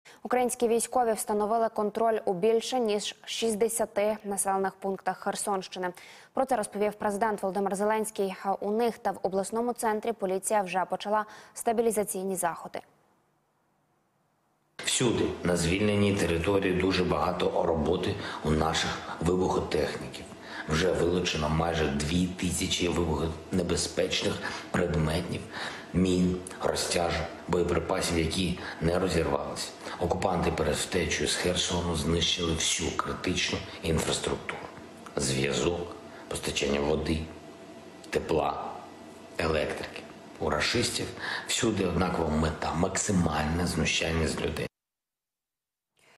На Херсонщині взято під контроль 60 населених пунктів — Зеленський
На Херсонщині взято під контроль 60 населених пунктів.